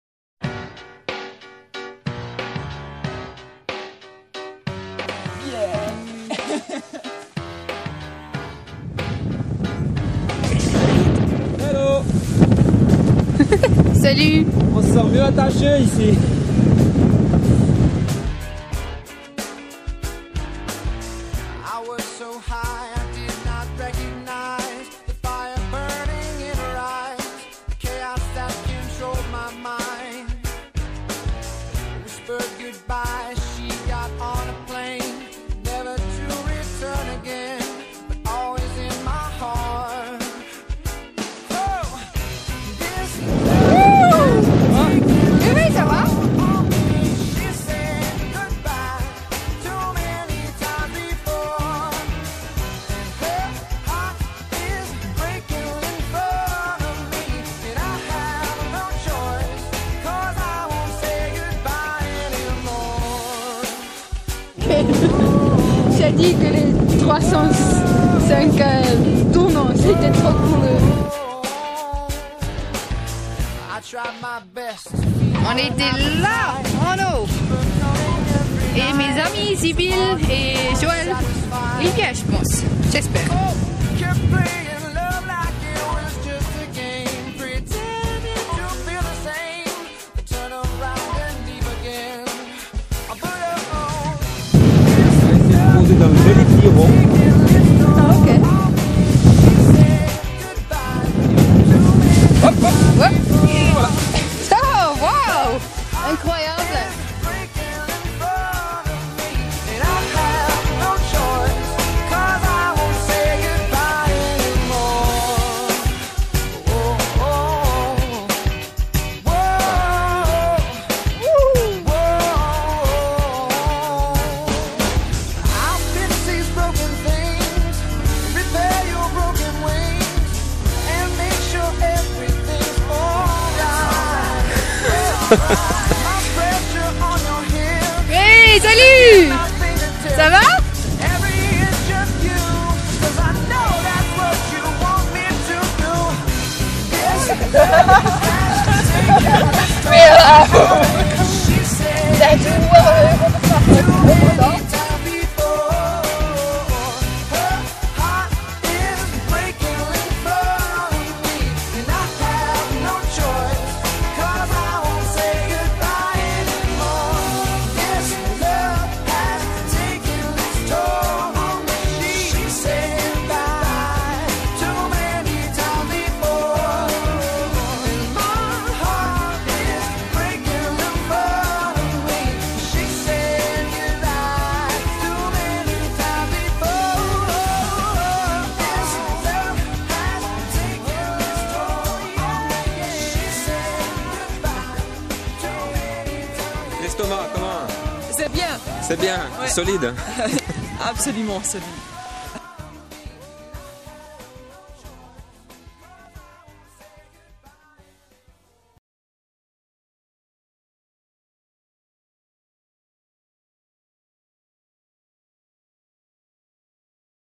Team Lipton en Parapente
Le Team marketing de Lipton découvre le parapente à Vercorin sous une voile aux couleurs Lipton